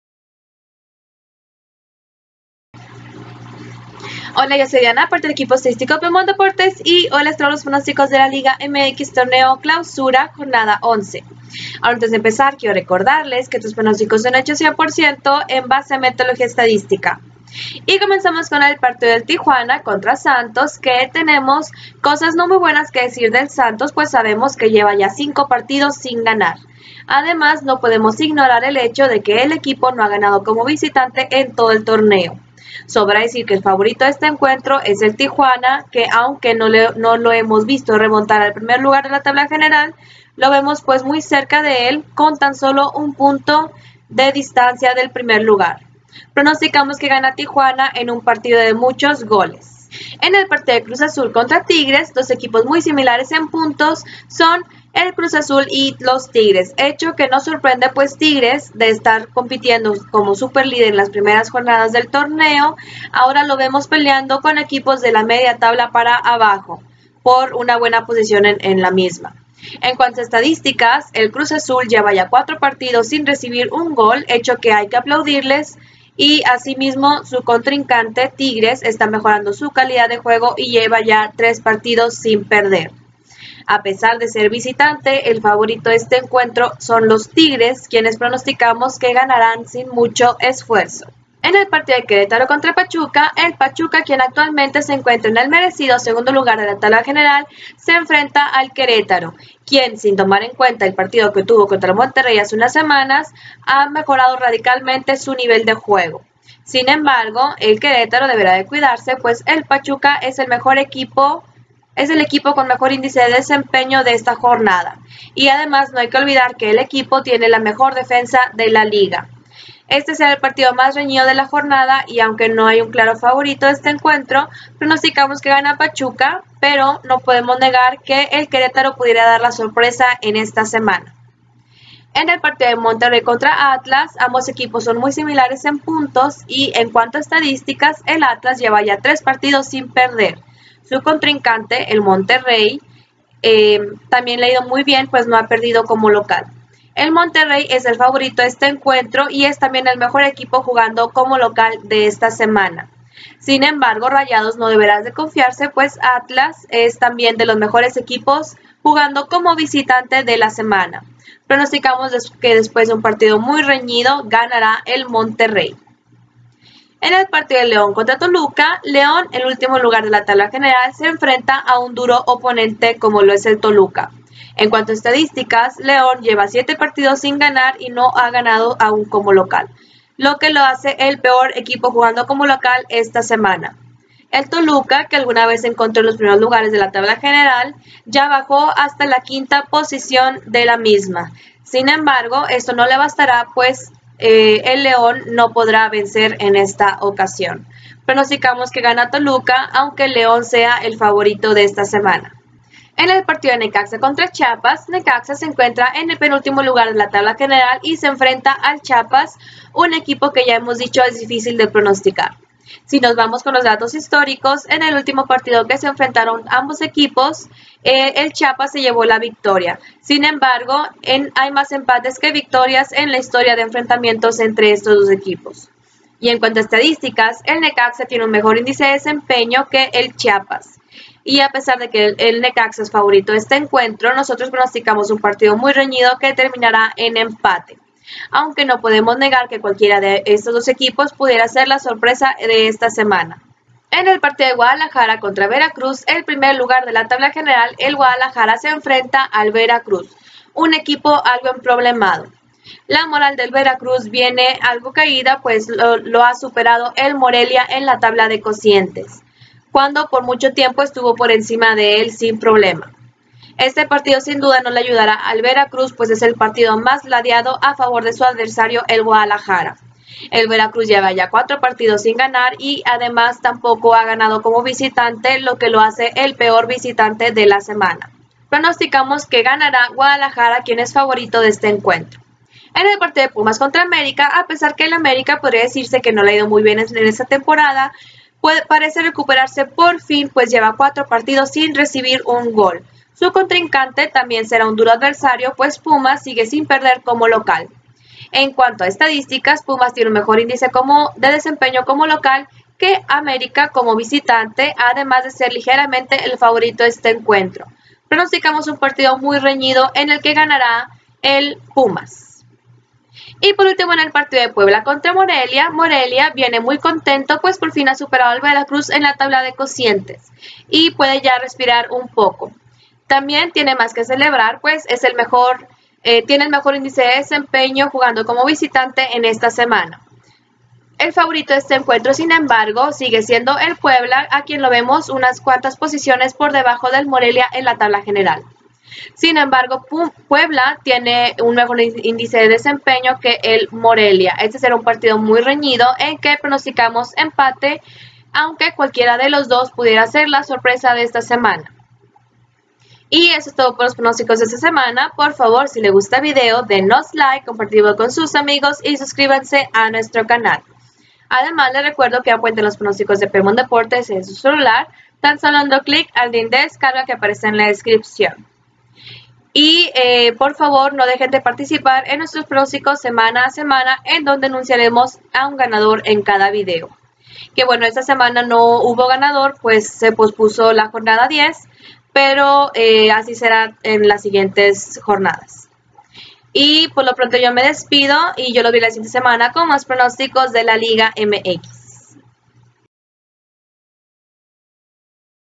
Pronósticos Liga MX Clausura 2017: Jornada 11
Te traemos los Pronósticos Liga MX de PebMont Deportes, correspondientes a la Jornada 11 del Torneo Clausura 2017.

Puedes bajar la aplicación de los Pronósticos PebMont dando